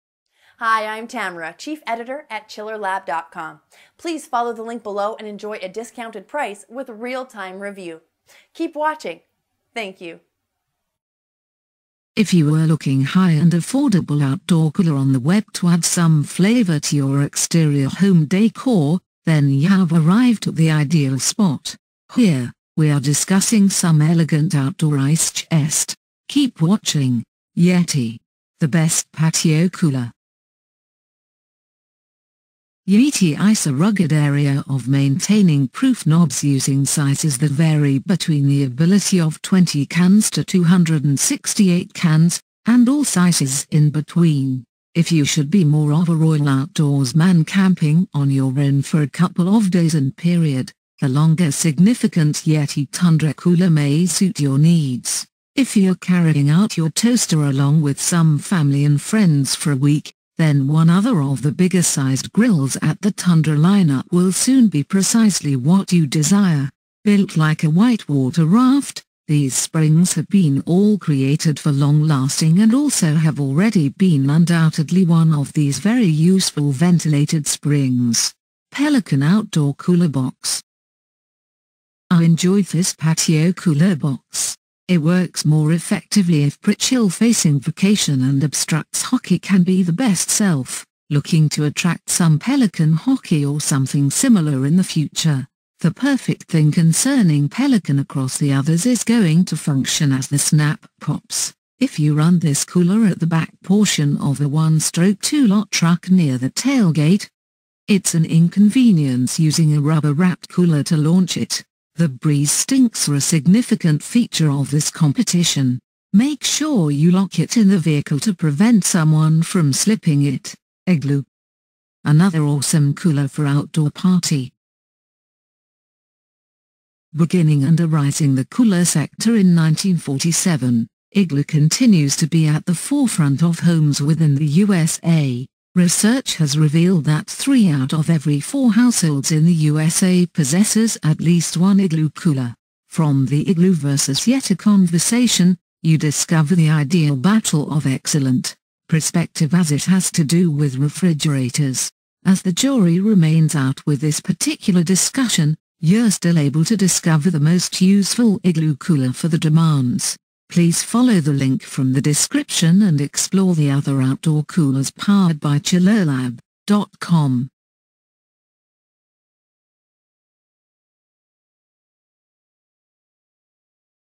Best outdoor Cooler Review, Coolers with wheel, Wooden Cooler and many more
If you were looking high and affordable outdoor cooler on the web to add some flavor to your exterior home decor, then you’ve arrived at the ideal spot.

Here, we are discussing some elegant outdoor ice chest. Keep watching.